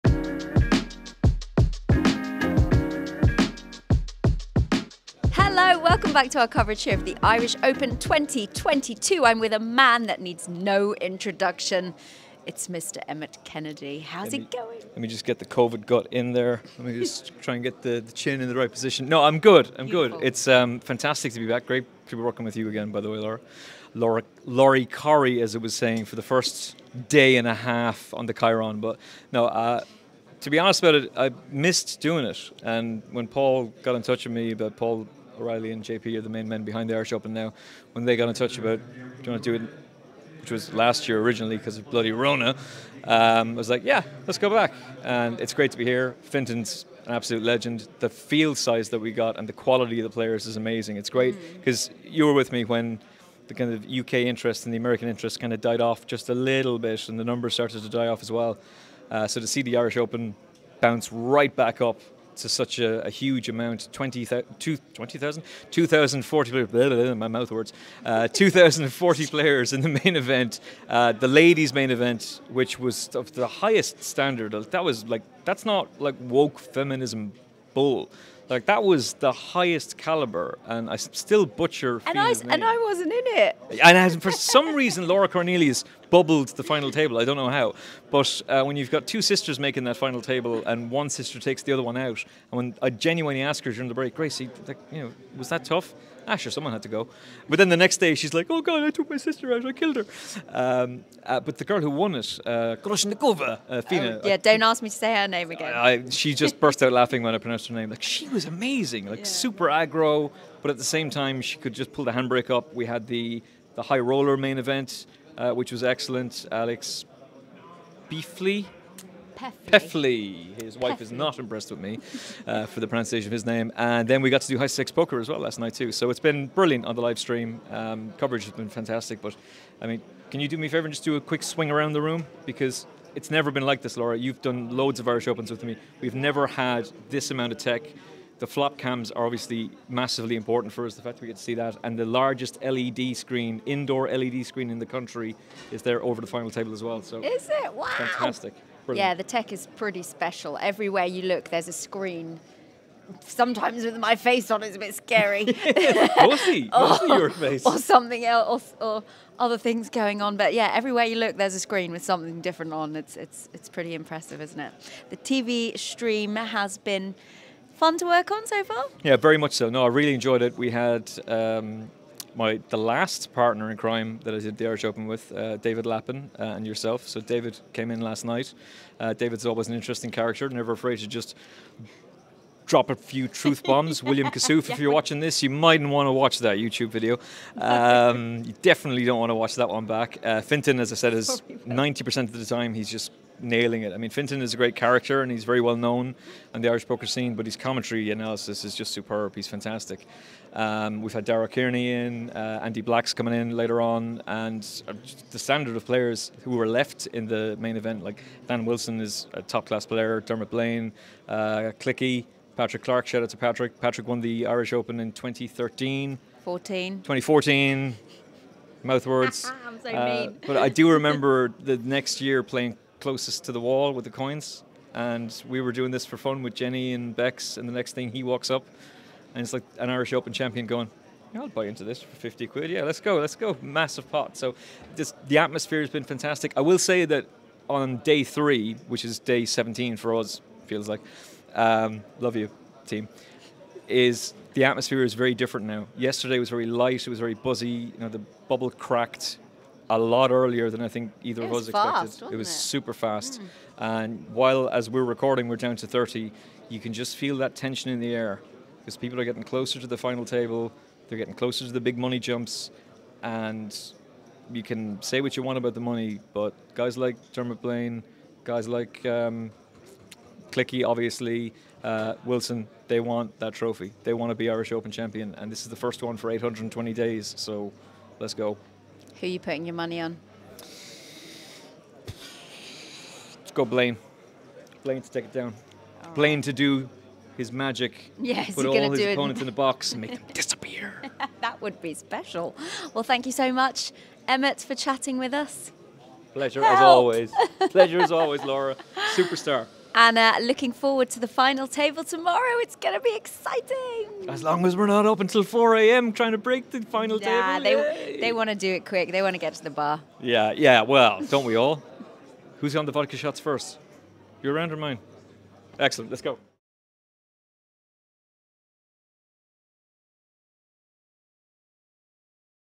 Irish Open 2022 - Emmet Kennedy
Laura Cornelius talks to Emmett Kennedy about getting back on the Irish Open comms with Fintan Gavin et al after two years.